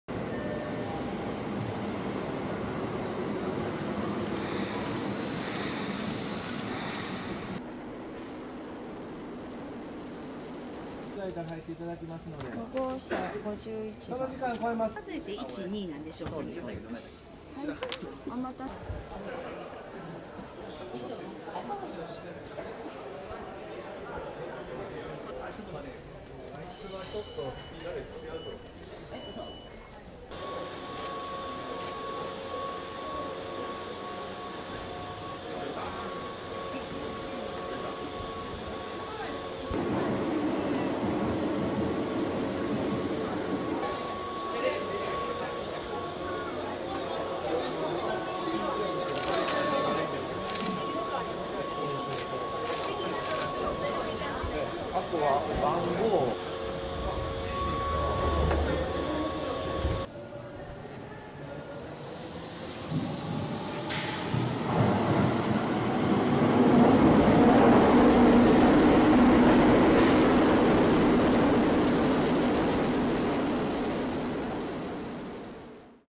京阪「テレビカー」ラストラン
京阪電車で長年親しまれてきた「テレビカー」が３１日、ラストランを迎えた。